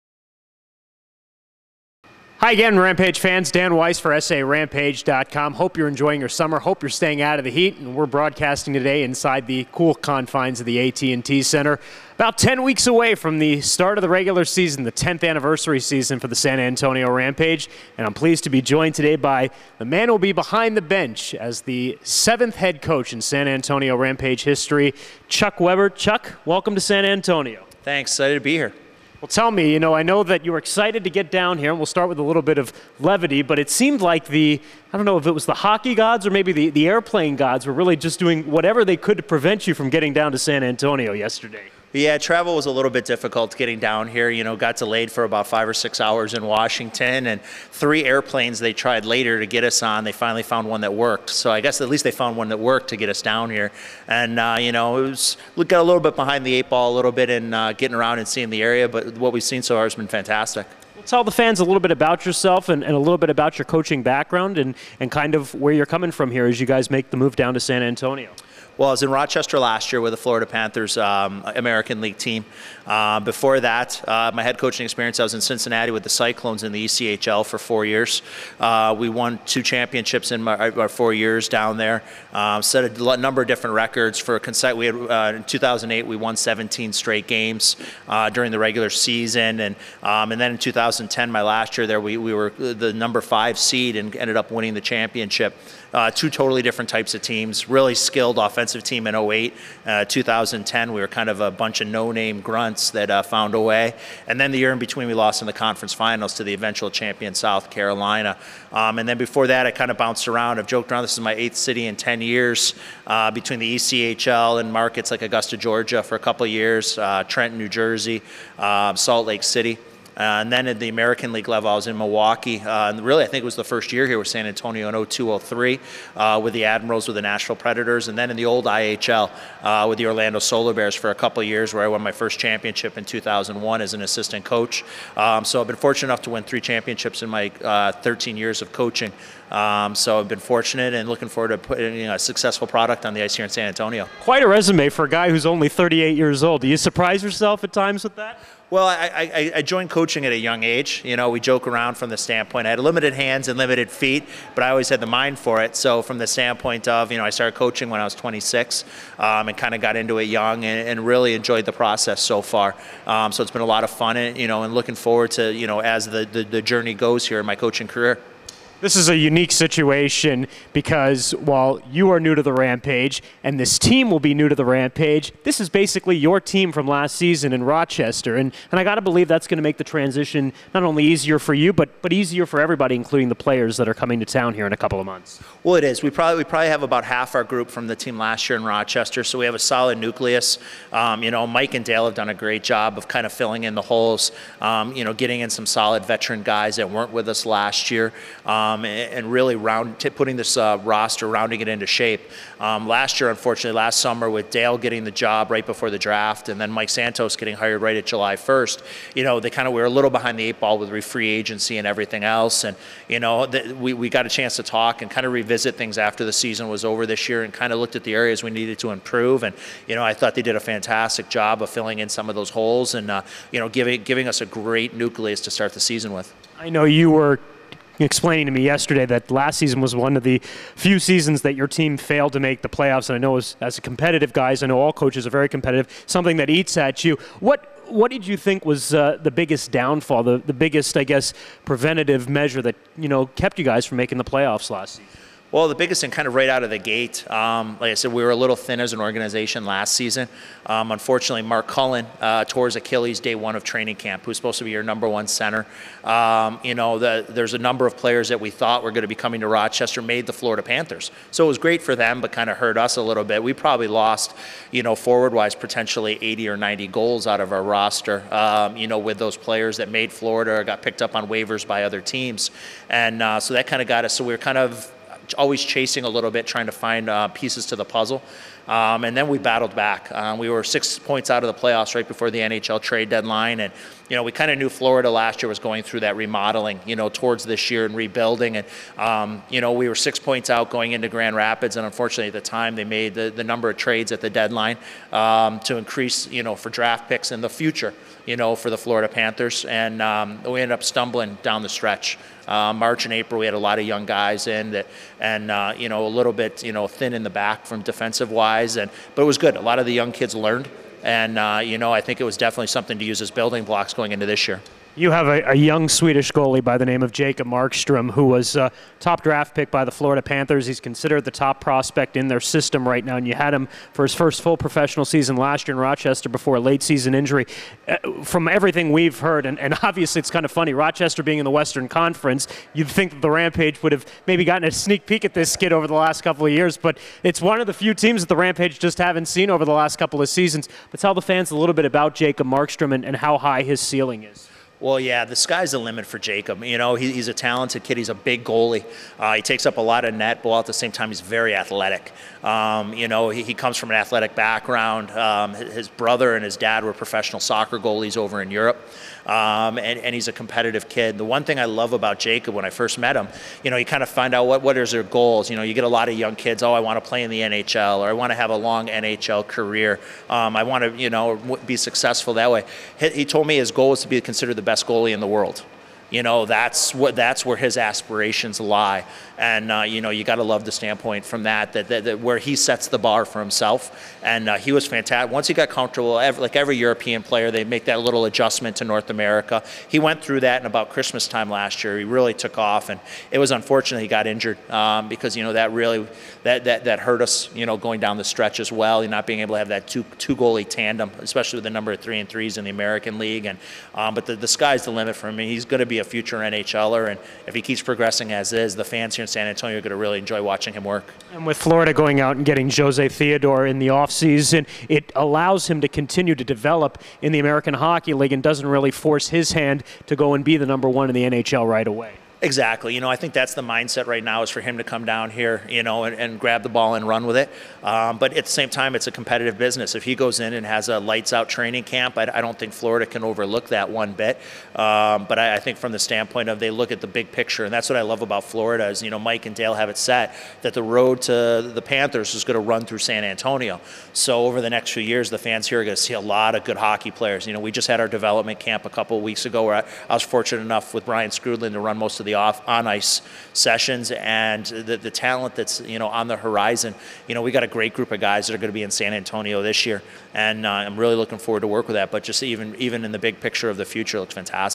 Interview with Rampage Head Coach Chuck Weber, Part 1
Voice of the Rampage Dan Weiss interviews Rampage Head Coach Chuck Weber on a variety of topics about the upcoming 2011-12 season.